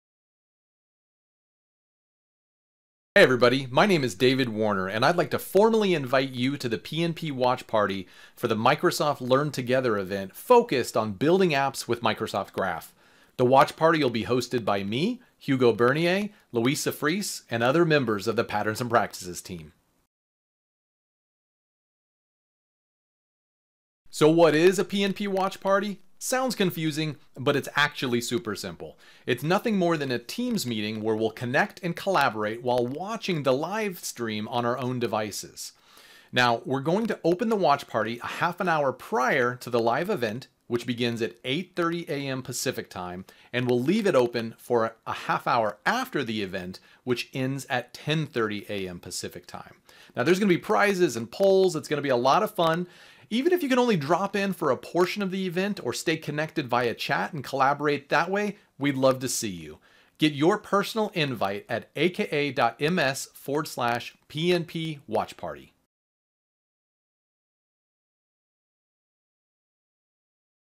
PnP Watch Party - Building Apps with Microsoft Graph - Learn Together
Join the PnP Watch Party for the Building Apps with Microsoft Graph - Learn Together event.

April 14th, 2021 - 8:30am PST

The watch party will open 30 minutes prior to the start of the event (8:00am PST) and will remain open for 30 minutes after the event.